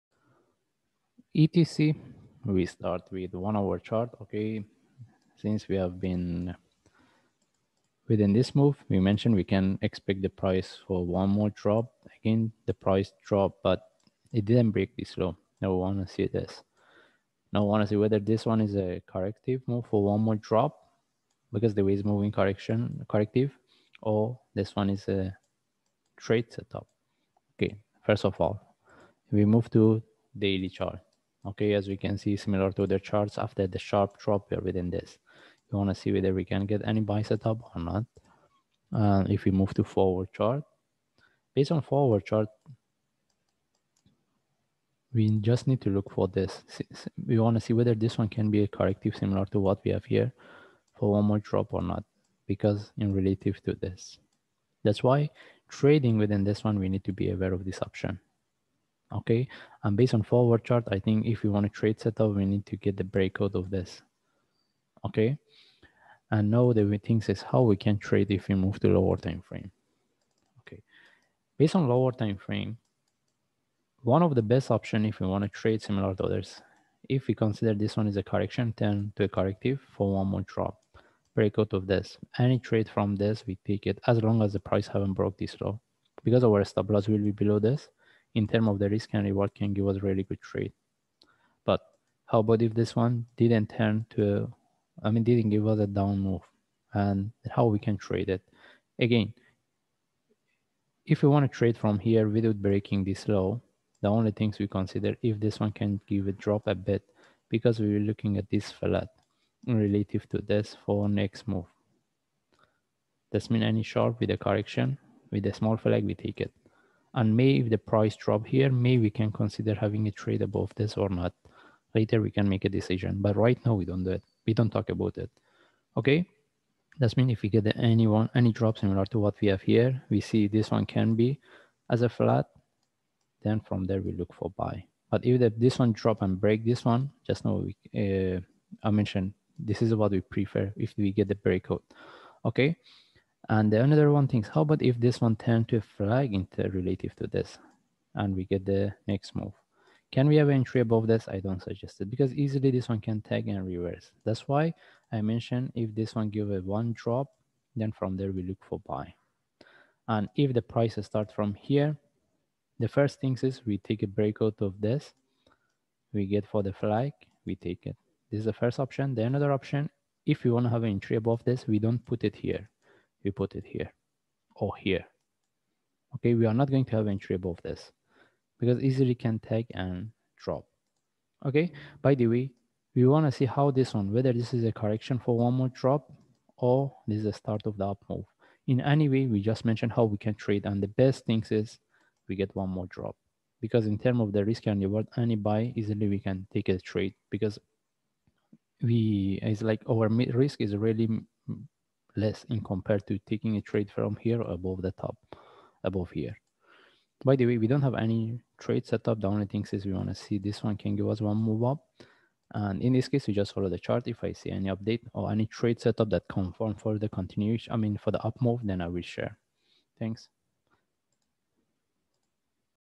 ETC - Ethereum Classic Technical Analysis for February 27, 2021 - ETC
Link for XRP:  Shows analysis for XRP and why we were expecting price to drop to 0.36